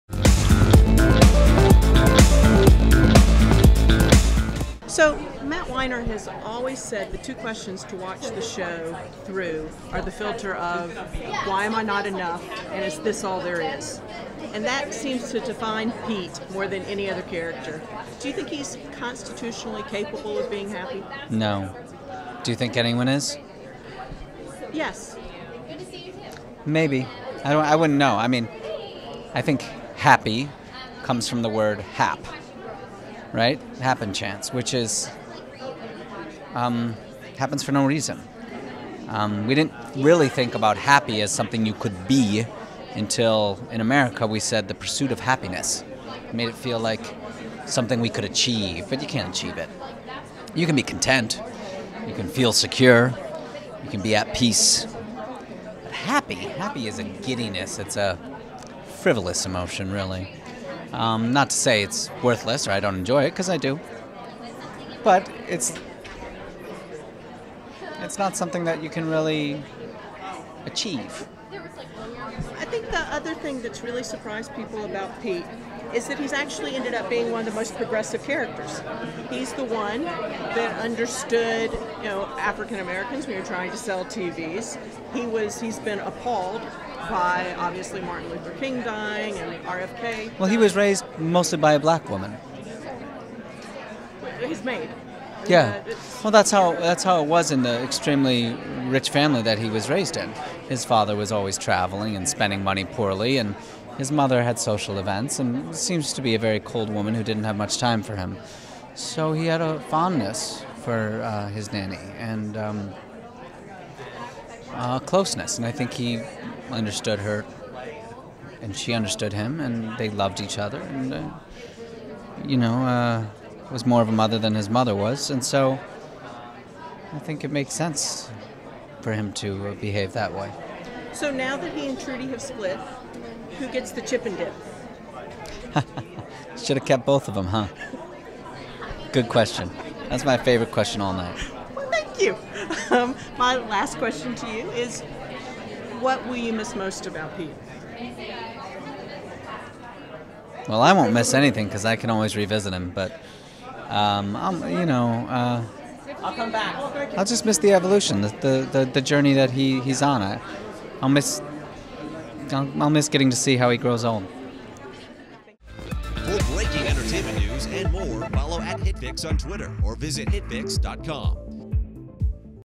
'Mad Men' star Vincent Kartheiser says he'll miss the evolution of Pete Campbell
HitFix caught up with Vincent Kartheiser at PaleyFest 2014.